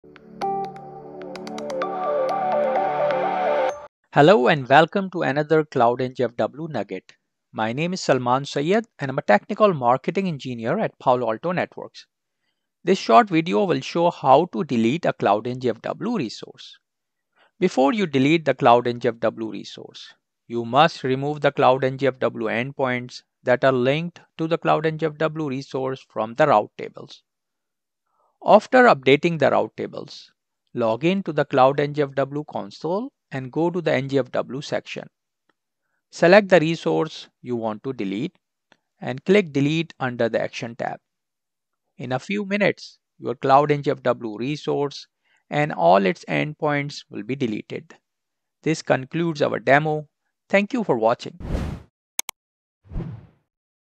Cloud NGFW for AWS: How to Delete Cloud NGFW Resource
Watch this how-to video to find out to delete the Cloud NGFW resource from the Console.